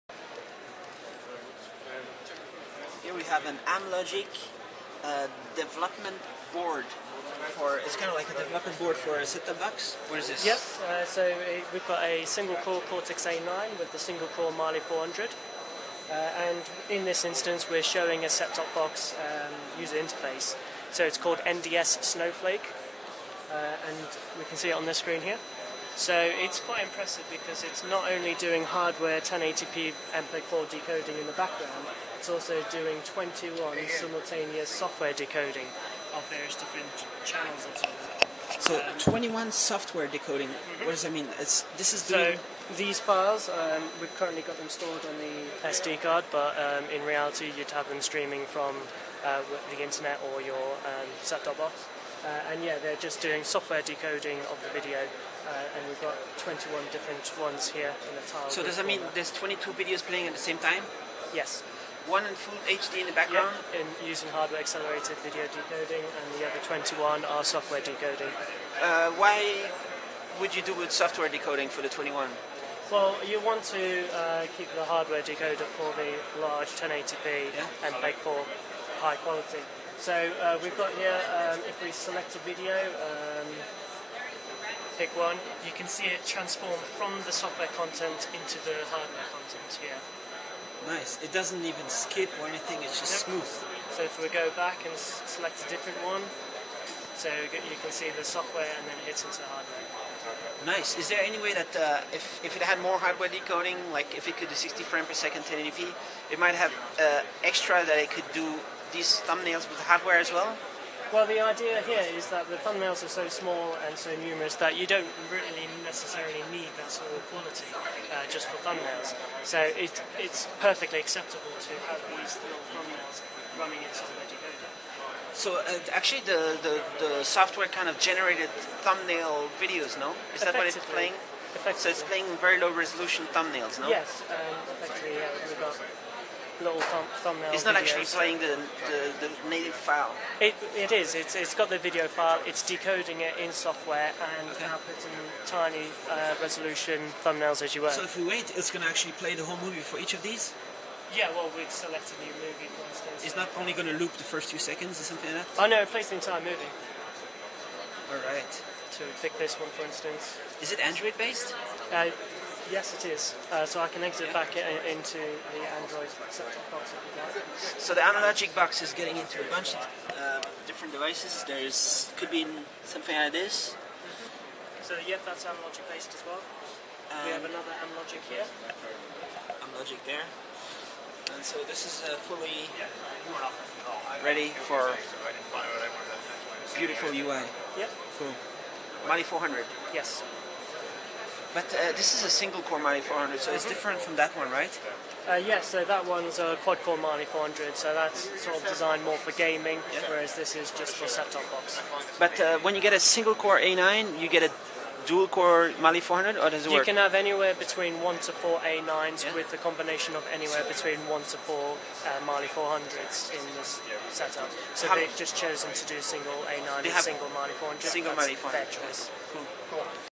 NDS Snowflake Set-top-box user interface demo
Using the AmLogic single-core ARM Cortex-A9 with Mali-400 graphics, here is a showcase of the NDS Snowflake Set-top-box user interface demo. NDS recently won an award for this ARM Powered Set-top-box user interface